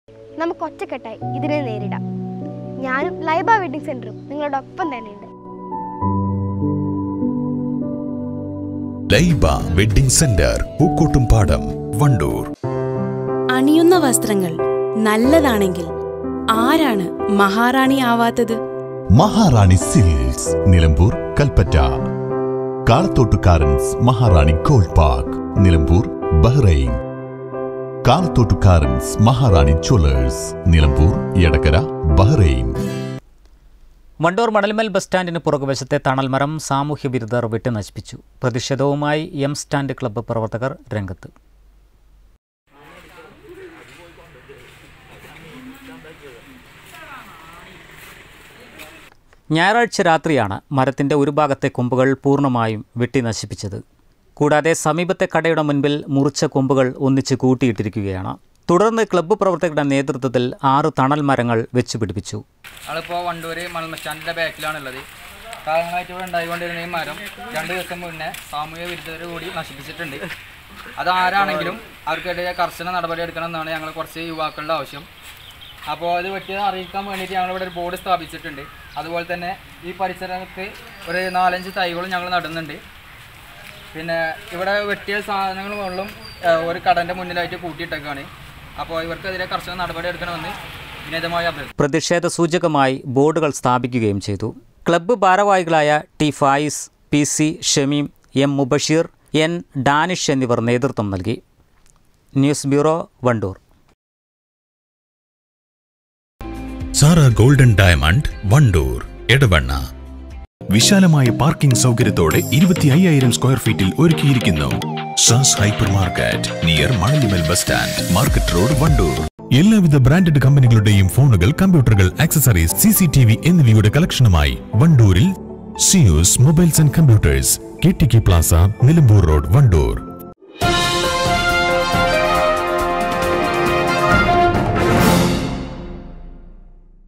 മണലിമ്മൽ പാടം ബസ്റ്റാന്റിന് സമീപത്തെ തണൽ മരം വെട്ടി നശിപ്പിച്ചു, പ്രതിഷേധവുമായി എം സ്റ്റാൻഡ് ക്ലബ്ബ്
മണലിമ്മൽ പാടം ബസ്റ്റാന്റിന് സമീപത്തെ തണൽ മരം വെട്ടി നശിപ്പിച്ചു,പ്രതിഷേധവുമായി എം സ്റ്റാൻഡ് ക്ലബ്ബ് പ്രവർത്തകർ.